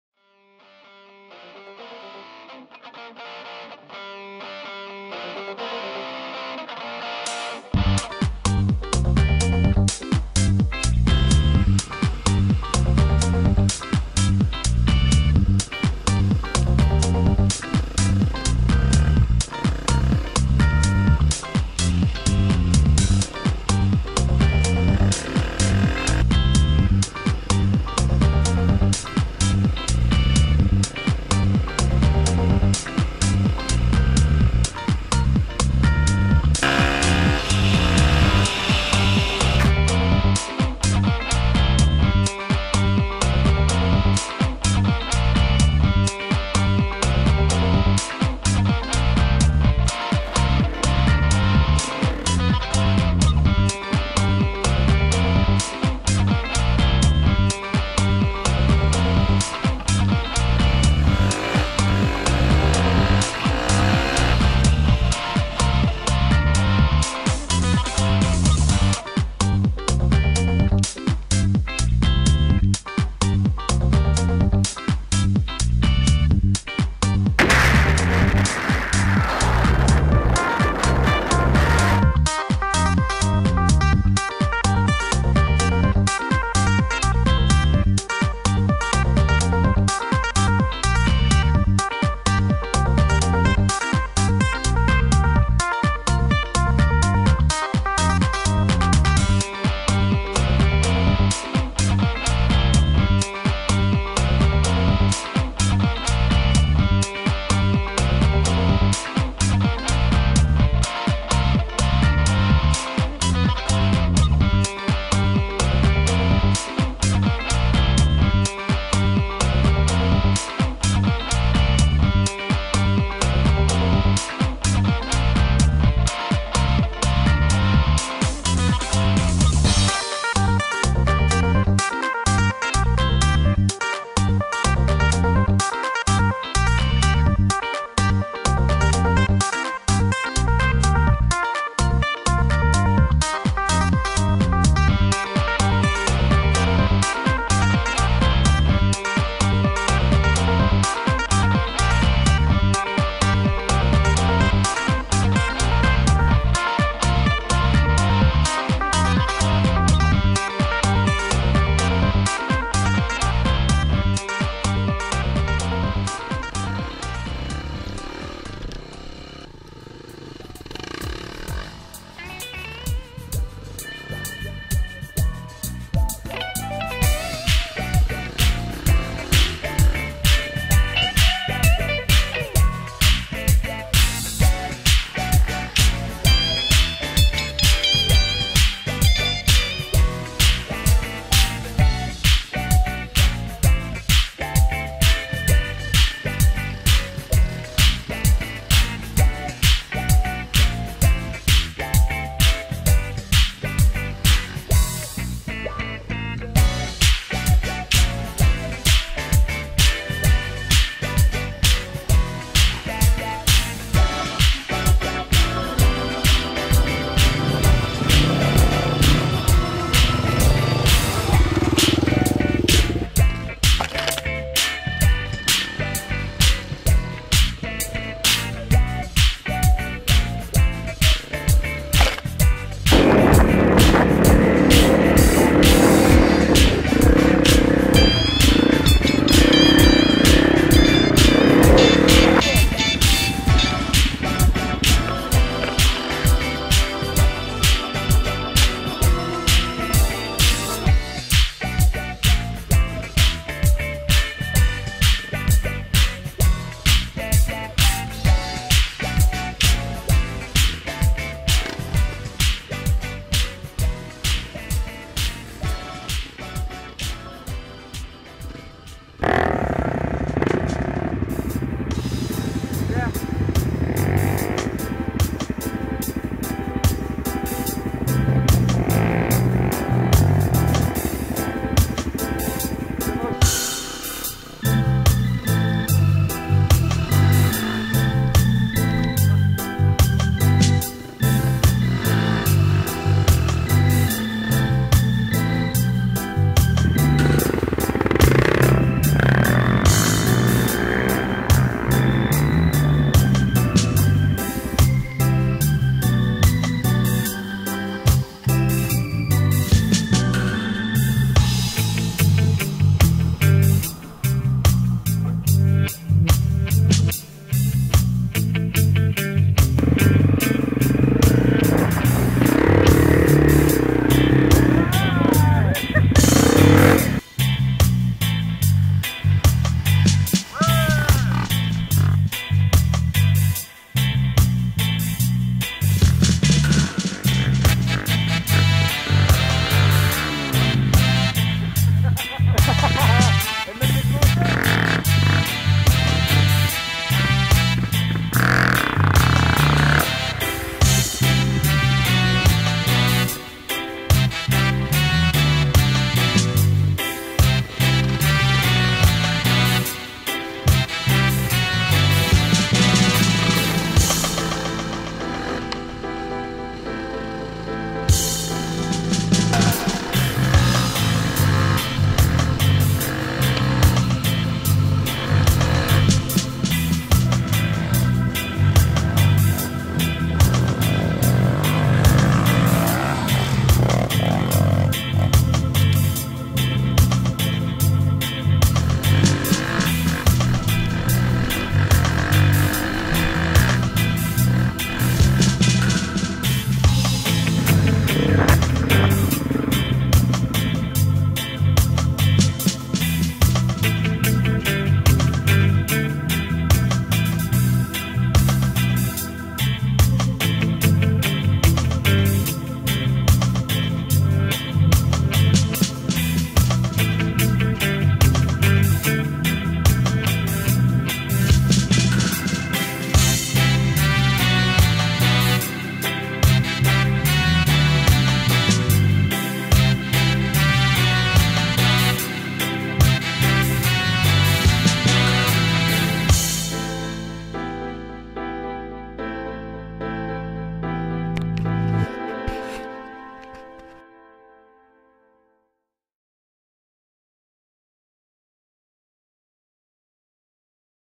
enduro pattaya decembre 2016 version finale low def
Sortie enduro à Pattaya 11 le décembre 2016